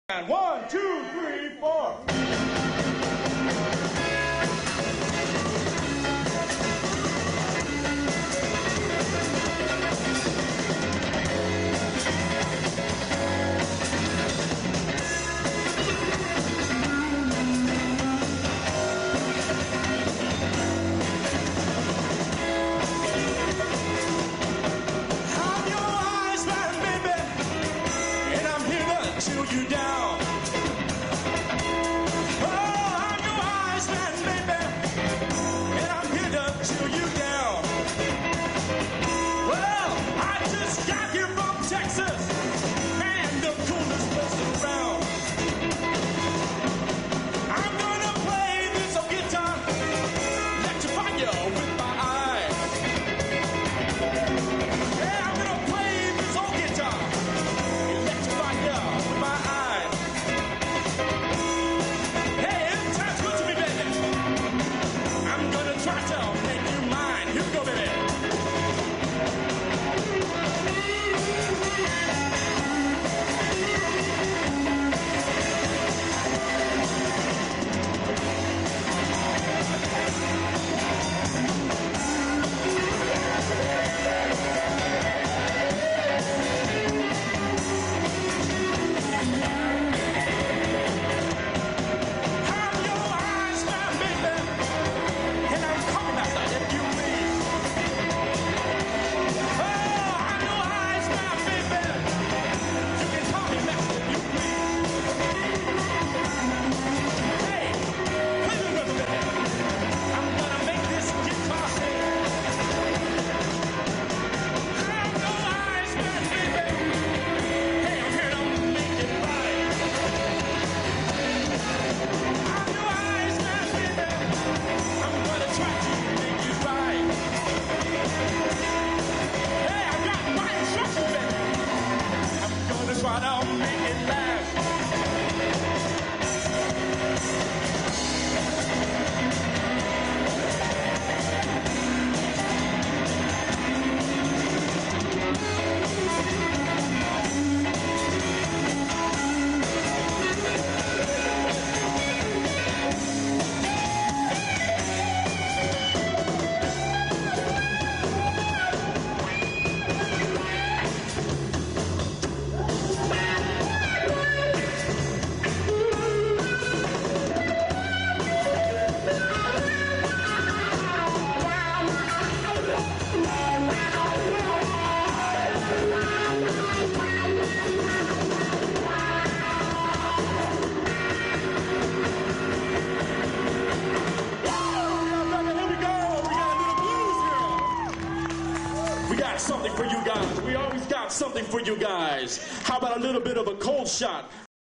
Tribal View Iceman
Battle of the Bands College of Lake County 1996 - Hey.  We Won!